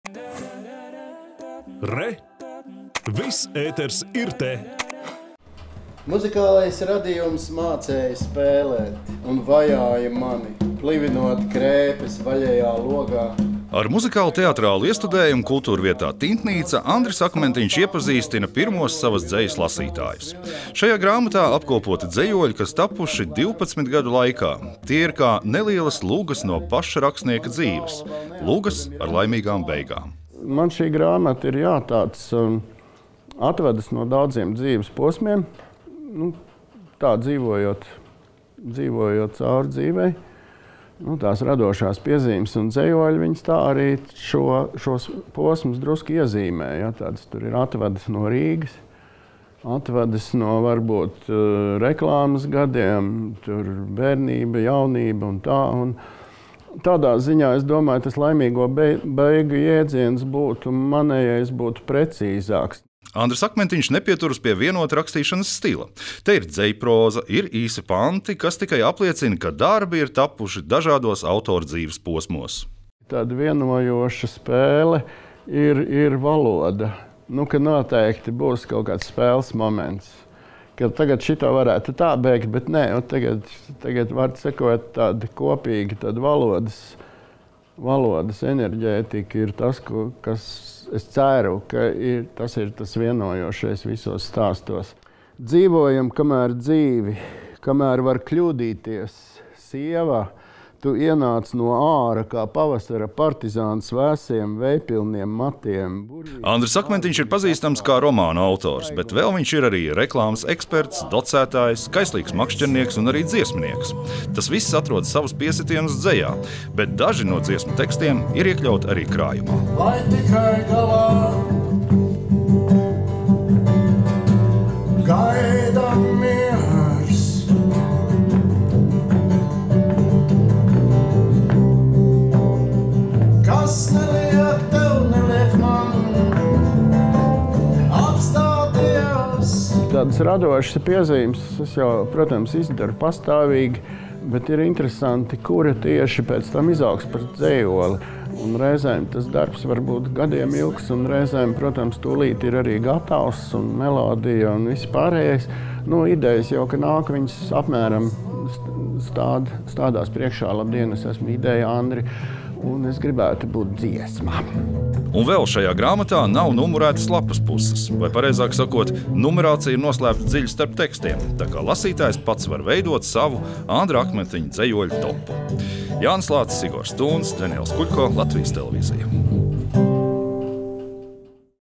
Laimīgas beigas Andra Akmentiņa dzejā
Tie ir dažādi dzīves posmi, dažādas attiecības un procesi, kas beigušies. Un, ja jau beigušies, tad laimīgi. Tā mans kolēģis Jānis Lācis raksturo rakstnieka Andra Akmentiņa dzejas grāmatu, kurai dots tieši tāds nosaukums – “Laimīgas beigas”. Krājums tapis daudzu gadu garumā un beidzot nonācis pie lasītājiem.